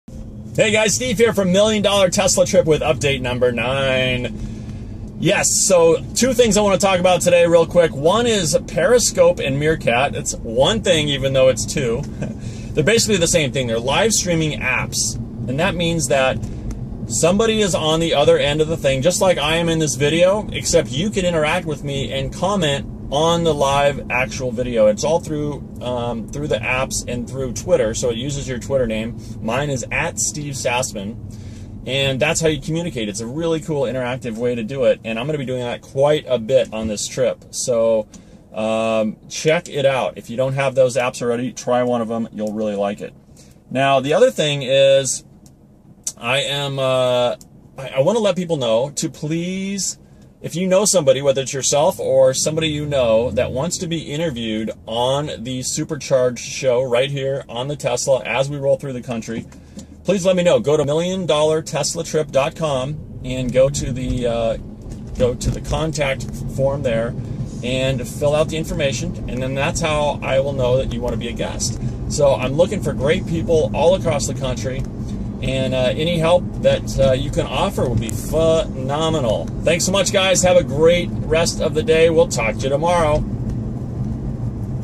Daily Update #9 - Meerkat & Periscope + How to be a Guest in Tesla
Check out the Live Streaming Apps on Meerkat and Periscope for your iPhone or Android (Meerkat only as of now) These are really interactive ways to follow this trip as well as discover lot's of interesting people around the world...and some weird ones too. It's the Internet after all...